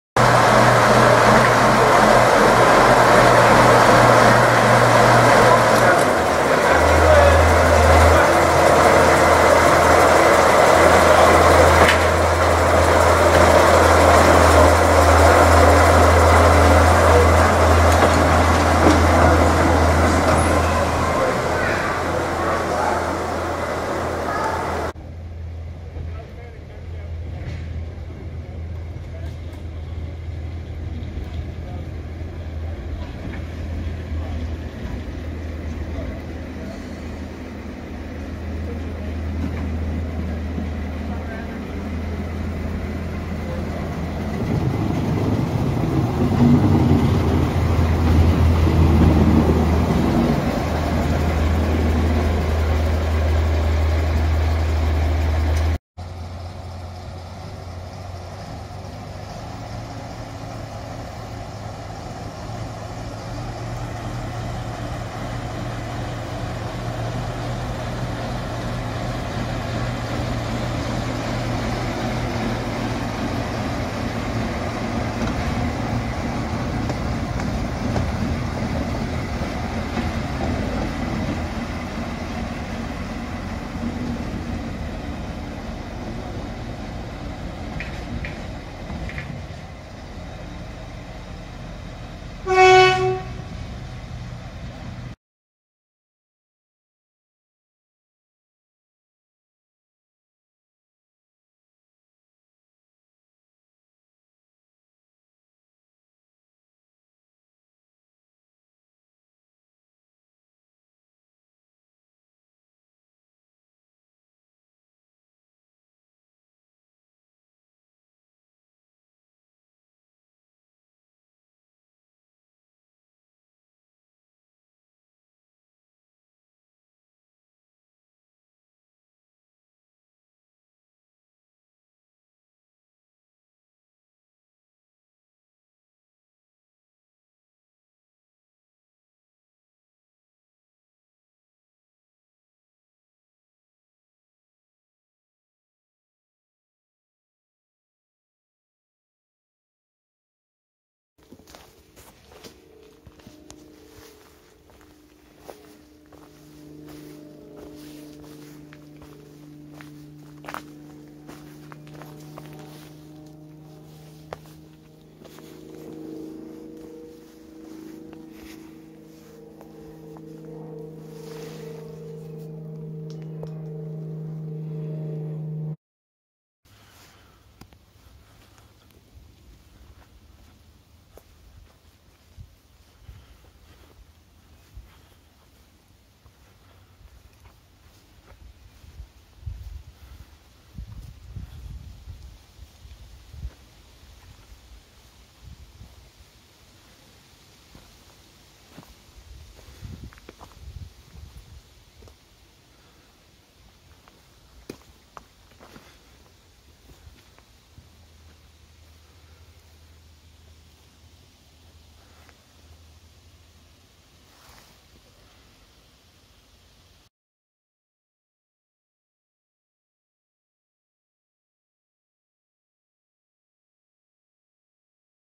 Sentinel / Rolls Royce 0-4-0DH 'BETTY' plus rolling stock pictures at Rocks by Rail 01/08/21
A few clips of Sentinel / Rolls Royce 0-4-0DH 'BETTY' plus pictures of various wagons and items of rolling stock which were on view.

Filmed on 1st August 2021 at Rocks by Rail (formerly Rutland Railway Museum), Rutland, England.